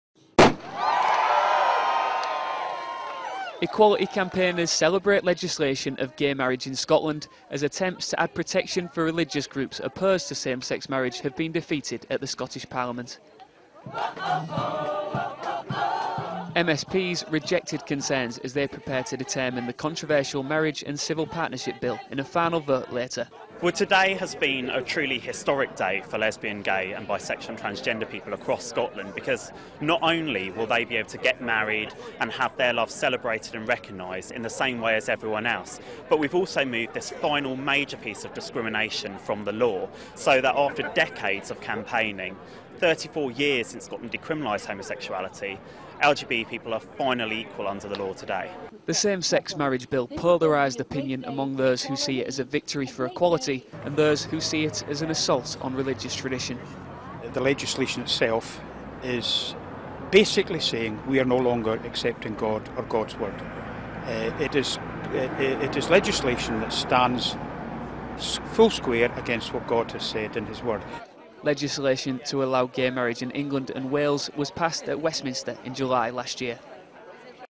Final Equality Network rally for equal marriage in Scotland
Footage from the final Equality Network rally for equal marriage in Scotland.

The Equality Network is a Scottish lesbian, gay, bisexual and transgender (LGBT) equality charity